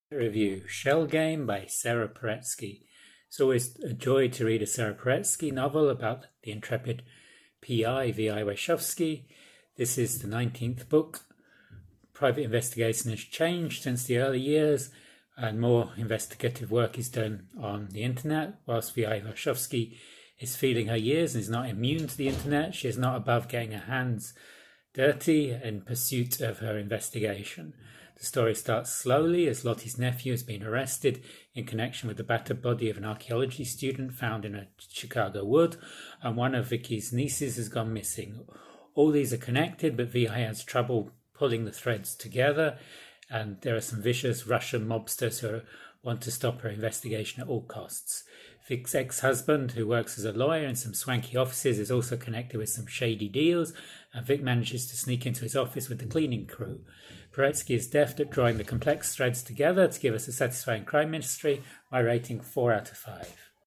1 minute Review: Shell Game by Sara Paretsky
This is a review of Shell Game by Sara Paretsky in around a minute.
This is the 46th one minute review I have posted, the first was another Sara Paretsky Novel, Dead Land.
Shell Game by Sara Paretsky is the 19th book featuring the interpid PI V I Warshawski.

One Minute Review No. 46

My Blog (for book reviews)
I read and answer all comments.